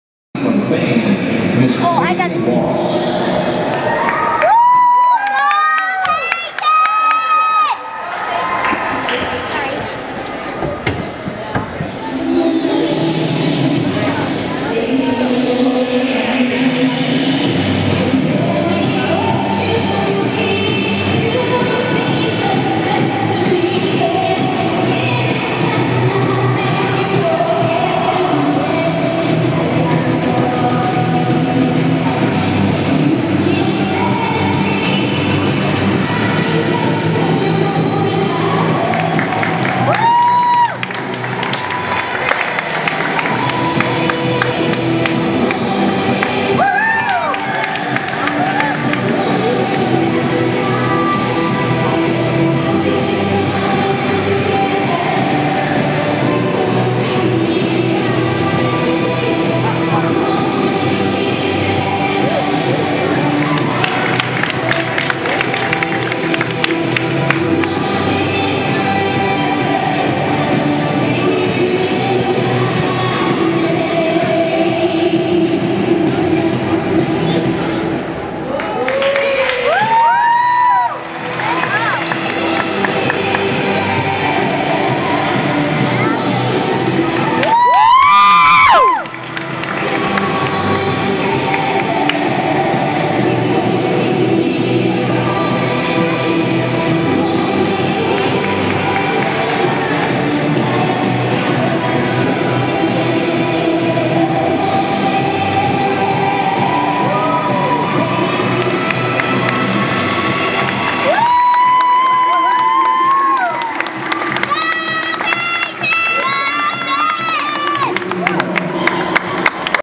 Peyton Fame Spring Show IEC May 2012
Peyton skates at the Independence Event Center Spring Show May 10, 2012